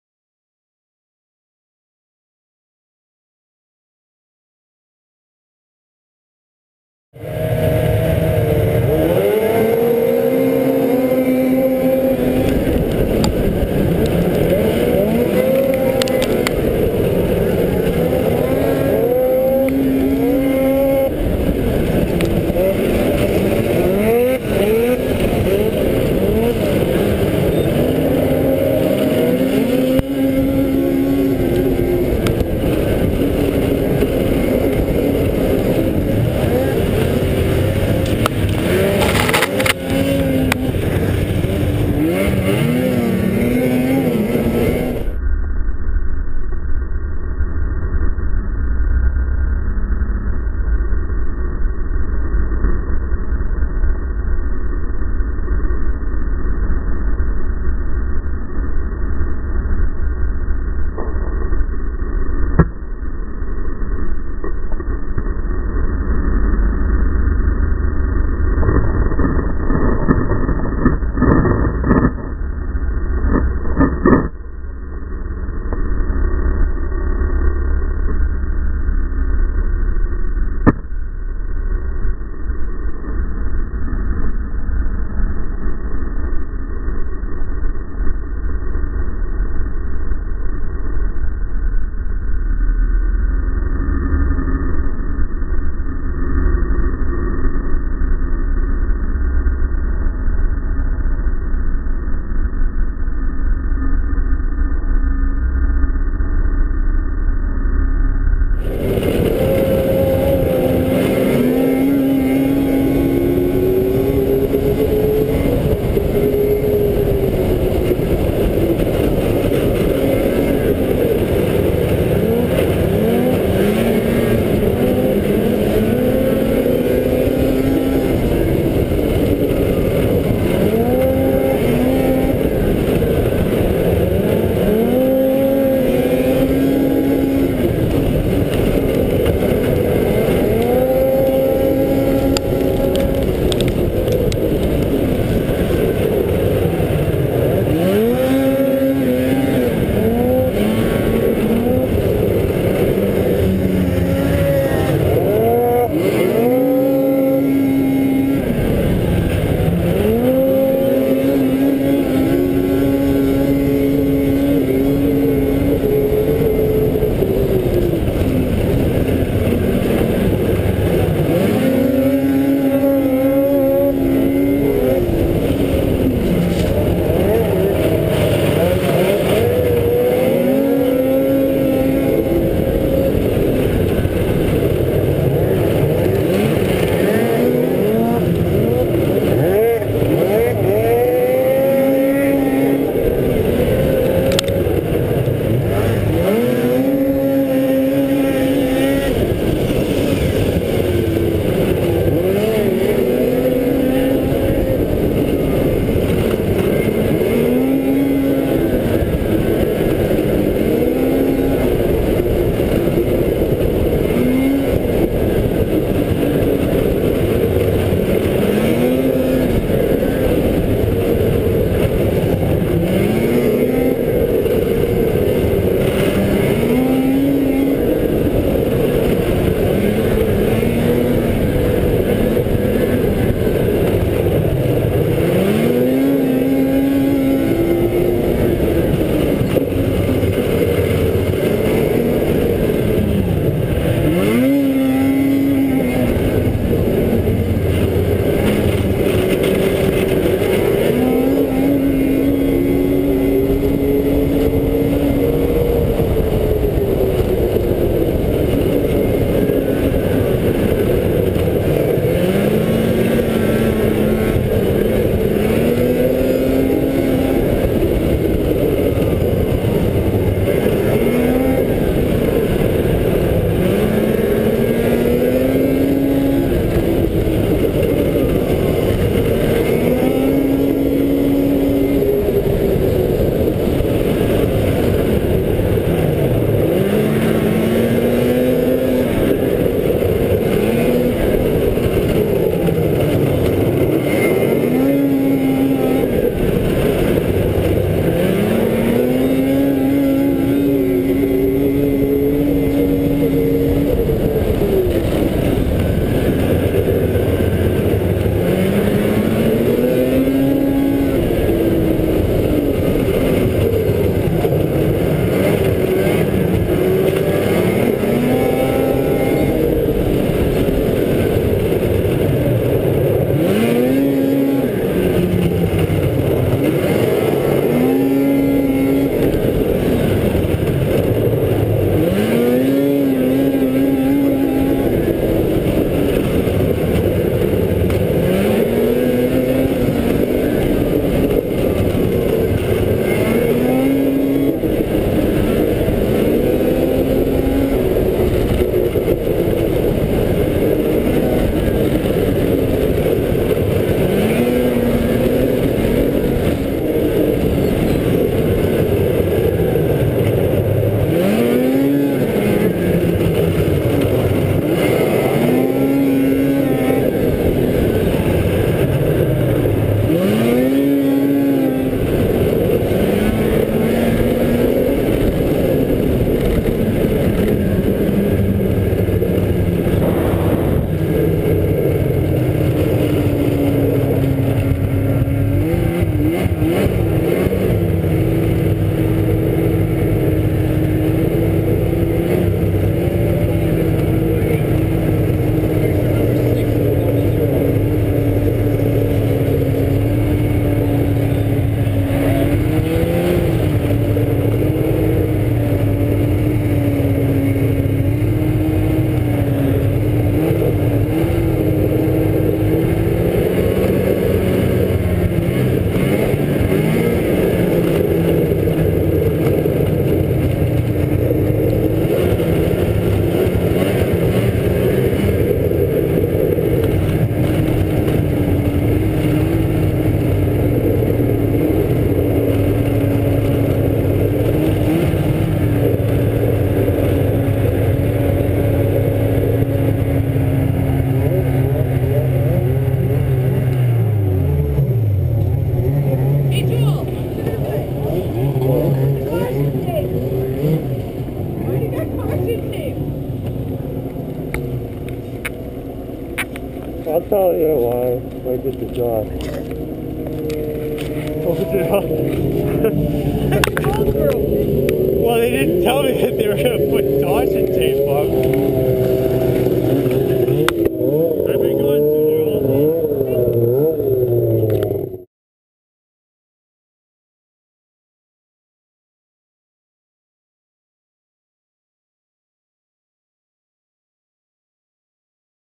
Taking Caution Tape Out!! snowmobile lakecross racing dyckesville WI 2-26-11
This is a helmet cam video of me snowmobile lakecross racing and taking the caution tape out in a pro open heat race.